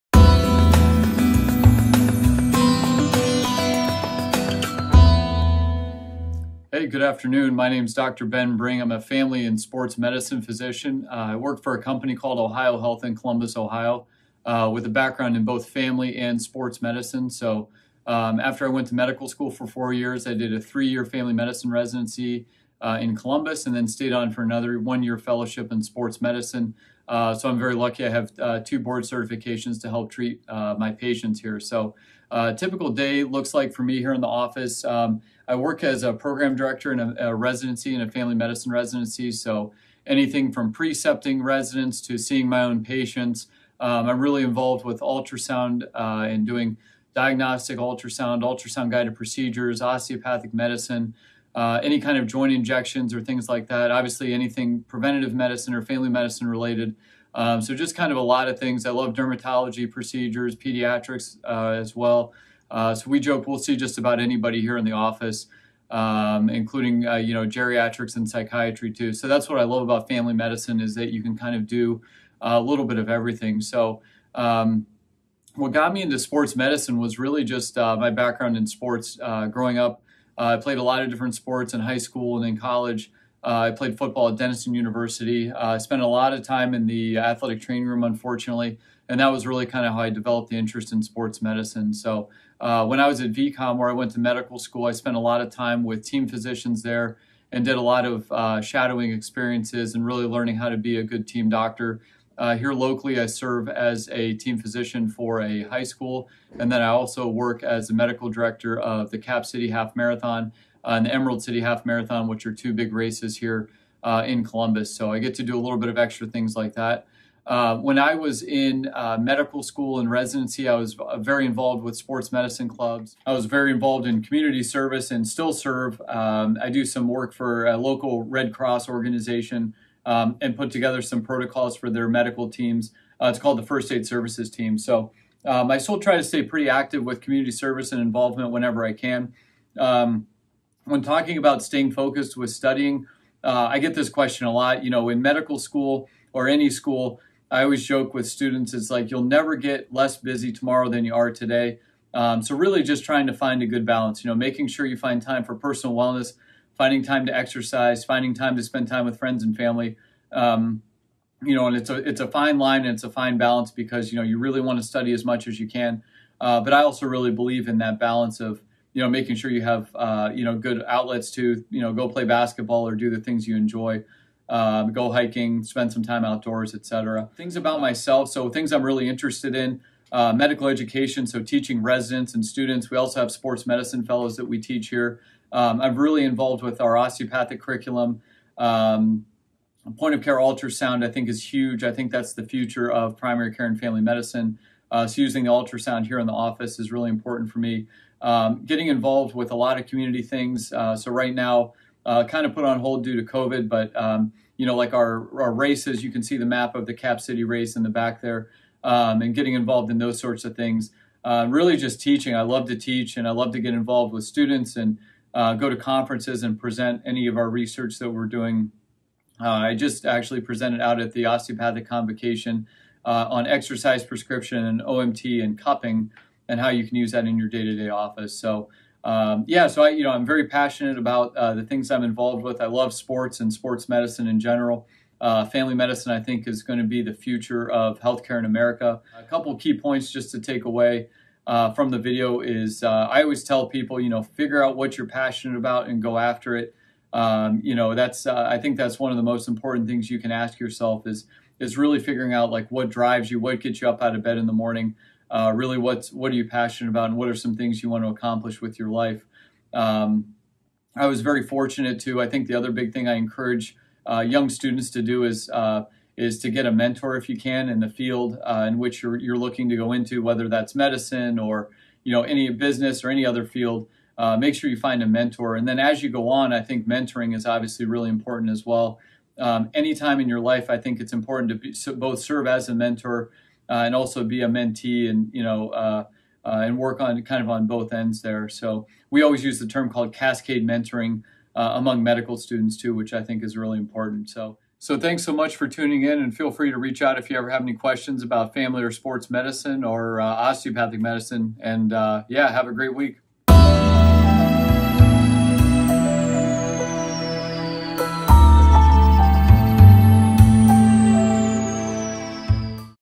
Benjamin Bring, Sports and Family Medicine Physician
Learn about becoming a Sports and Family Medicine Physician from an expert in the field!
for more great educational content on different careers and join an empowering network.